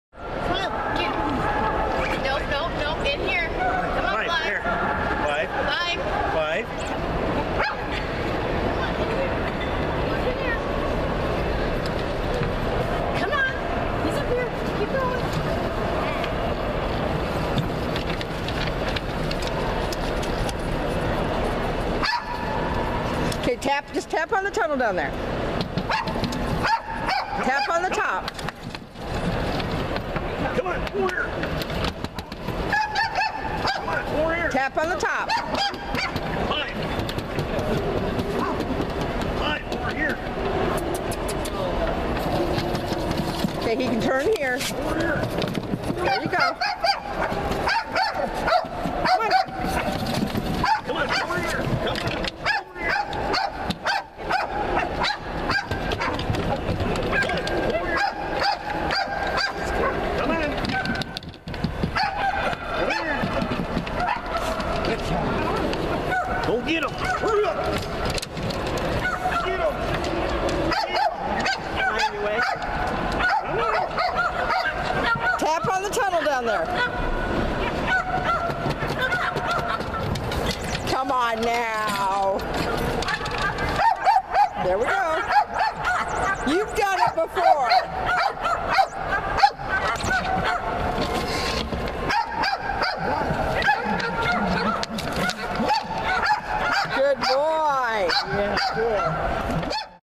Dandie Dinmont "Clive" in the Thunder Tunnel at the IKC
Video of a Dandie Dinmont terrier "Clive" in the the Thunder Tunnel at the IKC show in Chicago, IL January 2015.